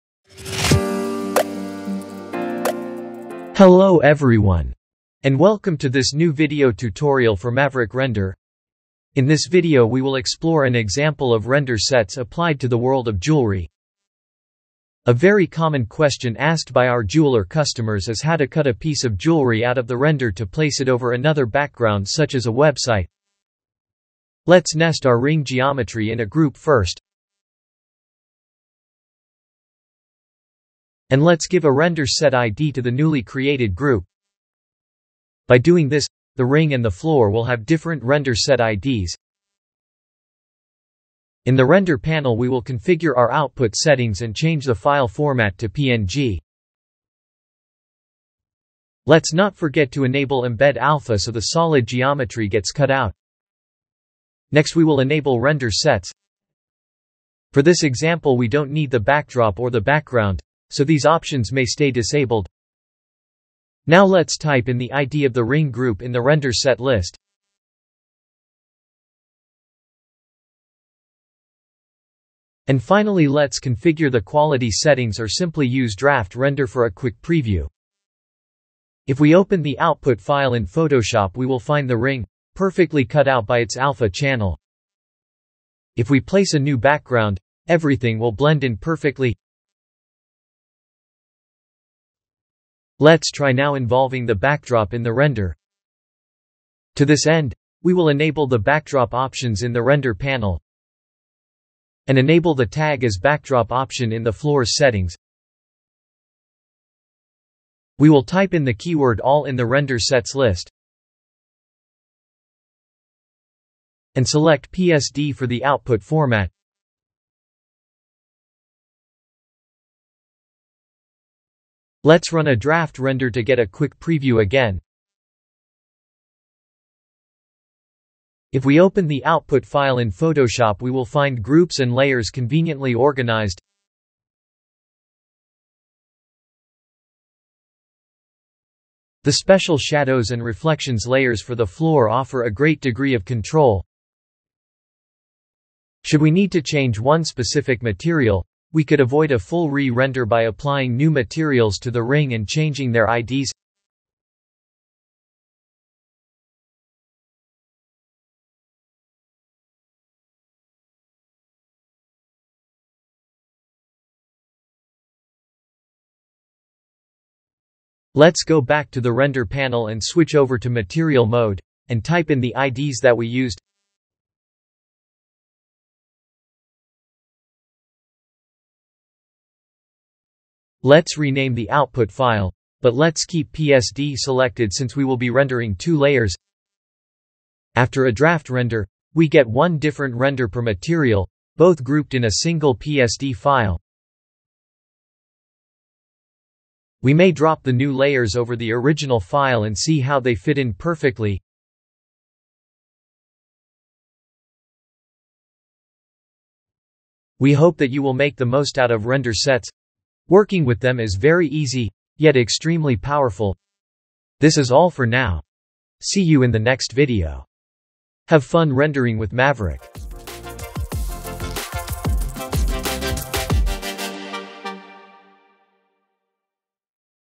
MAVERICK RENDER: 19 04 RENDER SETS: JEWELRY
In this video, we will go through an example case of use for Render Sets.
A typical jewelry sample: A ring over a background with shadows and reflections.
You will learn how to save a transparent png and a full PSD with the layers that you need for compositing.

Render Sets will allow you to render your scene detaching objects or materials into separate layers, and create a PSD file for compositing of said layers in an external app such as Photoshop.